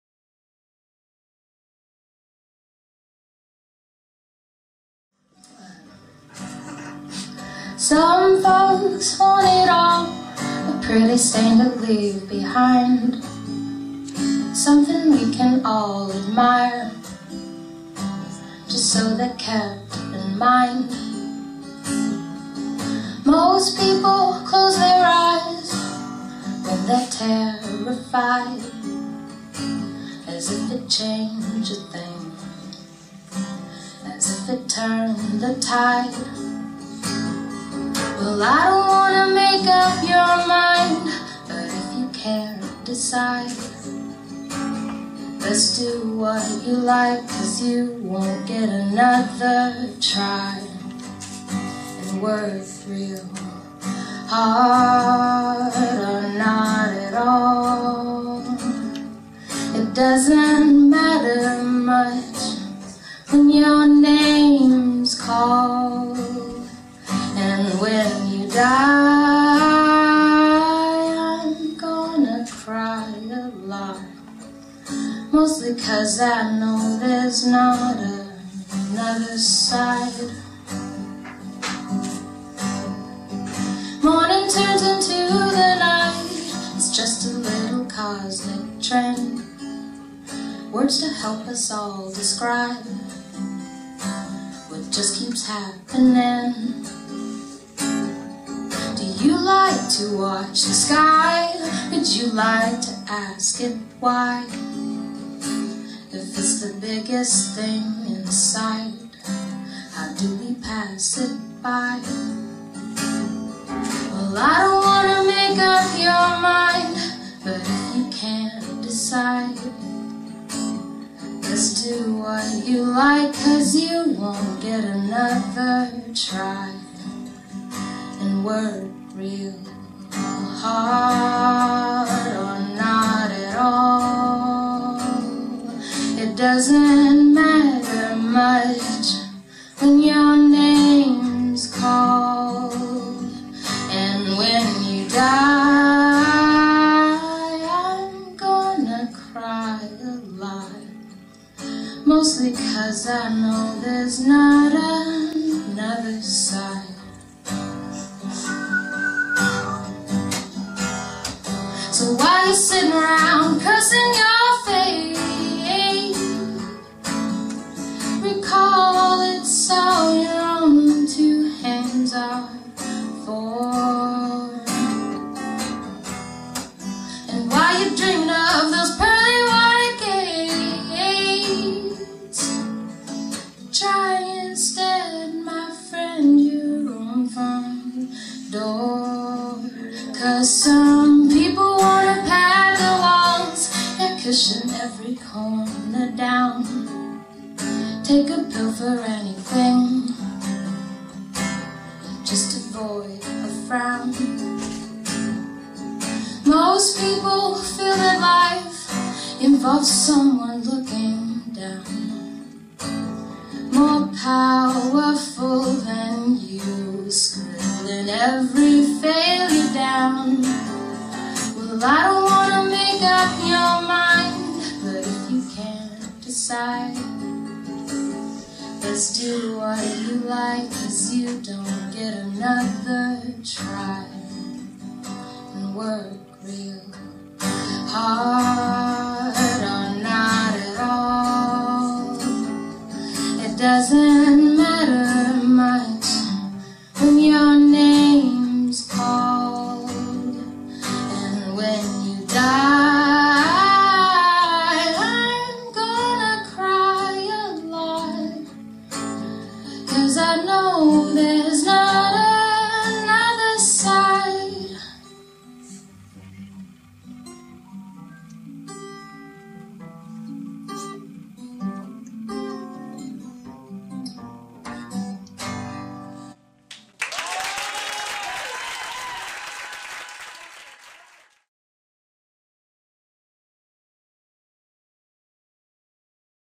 Holly Figueroa 01 - Maximum Ames Music Festival
Holly Figueroa performing at the Orange Gentleman during the Maximum Ames Music Festival.